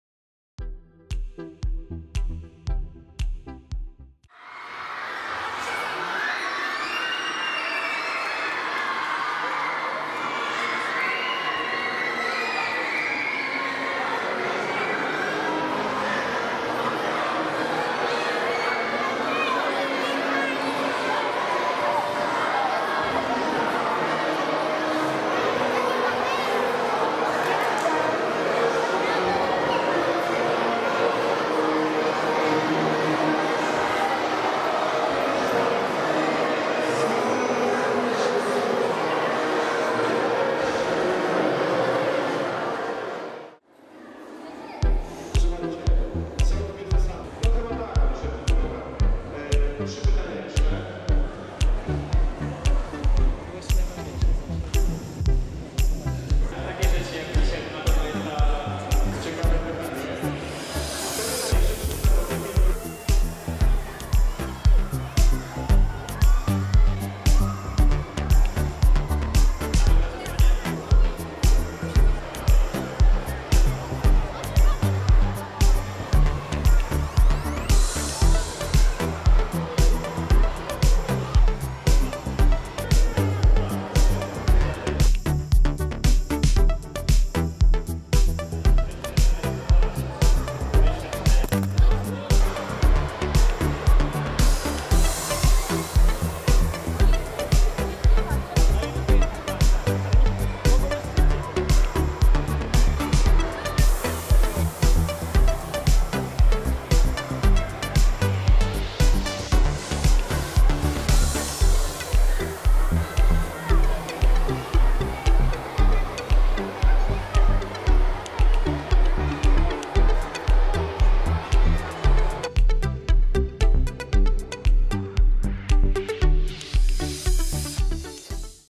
Jaga dzieciom: M. Wasiluk i D.Szymonowicz w SP nr 44
W środę piłkarze Jagiellonii - Marek Wasiluk i Dawid Szymonowicz udali się z odwiedzinami do Szkoły Podstawowej nr 44 w Białymstoku. Na miejscu naszych zawodników przywitało ponad 600 rozentuzjazmowanych dzieci.